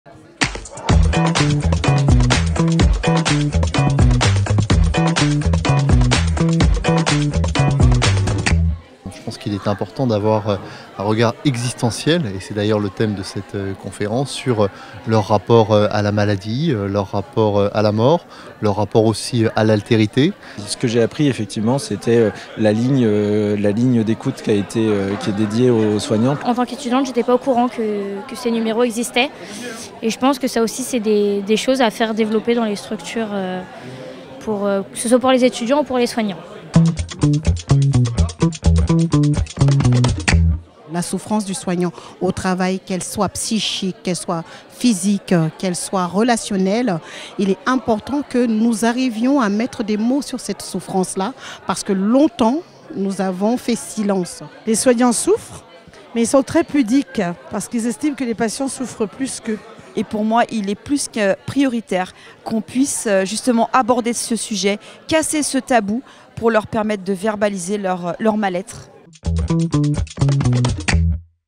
Clinique du Perreux | Questions/Réponses
Le mardi 18 avril 2023 a eu lieu la 1ère rencontre "regards croisés" à la Clinique SMR du Perreux, sur la souffrance existentielle des soignants.

La parole à...

Mathieu Lefèvre – Député de la 5ème circonscription du Val de Marne
Alexandre Lechevalier – Directeur Clinique SMR Inicea Val de Seine
Camille – Elève Infirmière
Colombe – Infirmière en Clinique SMR Inicea
Christiane – Assistante sociale
Christine Domingues - Adjointe de direction, Clinique SMR Inicea du Perreux sur Marne